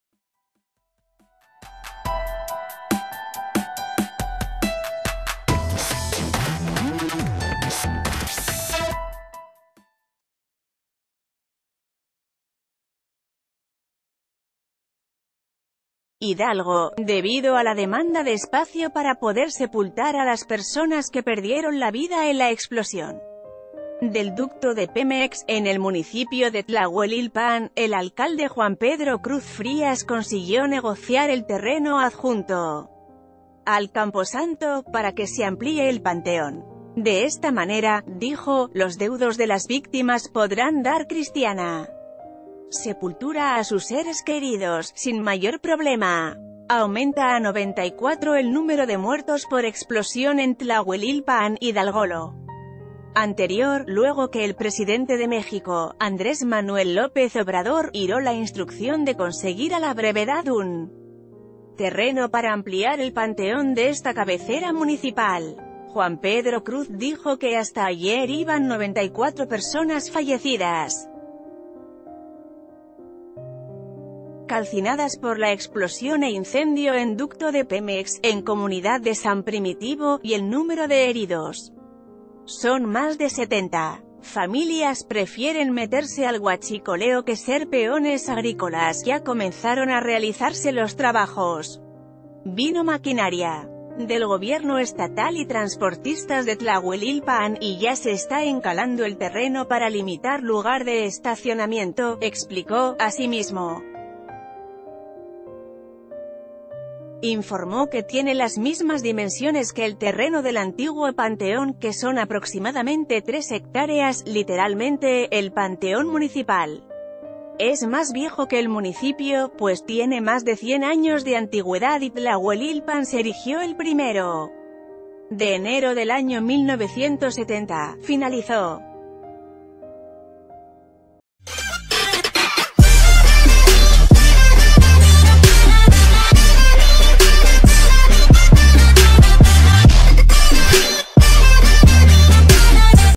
Amplían panteón municipal de Tlahuelilpan
Hidalgo.- Debido a la demanda de espacio para poder sepultar a las personas que perdieron la vida en la explosión del ducto de Pemex, en el municipio de Tlahuelilpan, el alcalde Juan Pedro Cruz Frías consiguió negociar el terreno adjunto al camposanto, para que se amplíe el panteón. De esta manera, ...